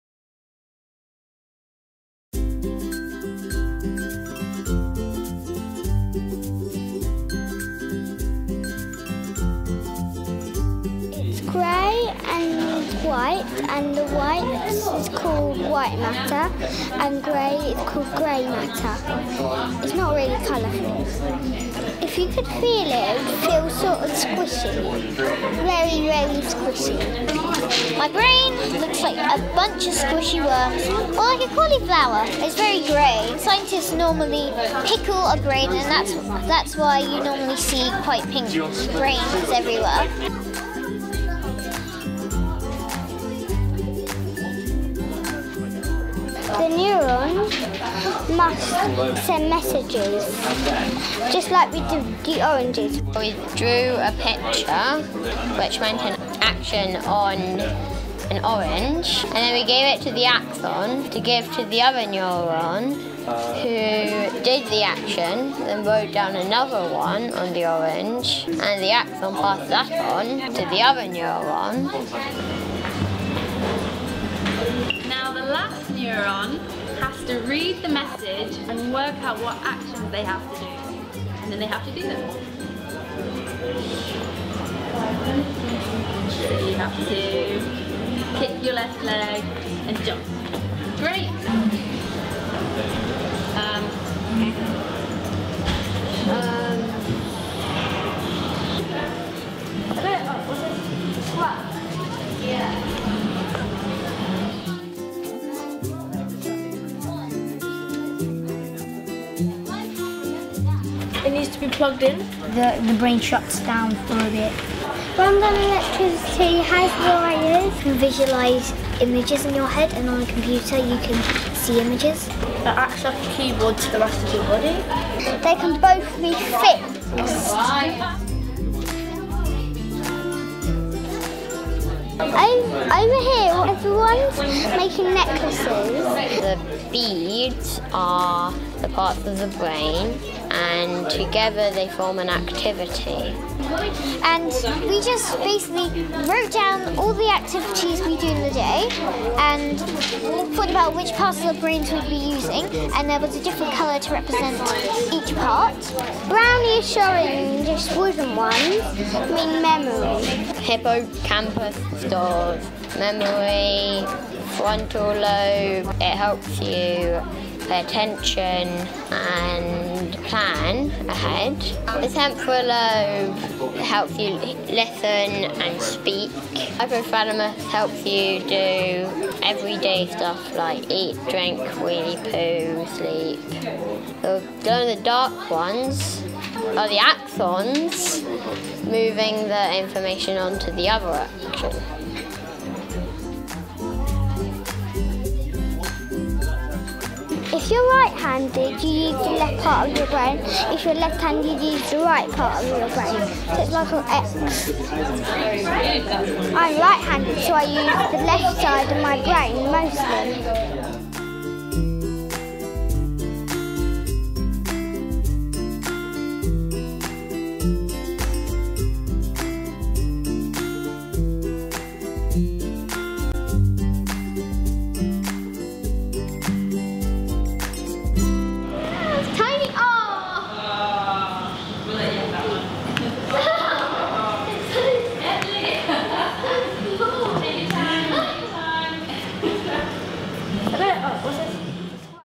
Meet Your Brain!
An educational short film made for, and presented by, primary school children.  Enjoy four fun minutes crammed full of interesting facts from the world of neuroscience!

Produced and directed by Piers Bailey, in collaboration with London Brain Project.

Funded by Great Ormond Street Hospital Biomedical Research Centre and NHS National Institutes for Health Research as part of the London Science Festival 2014.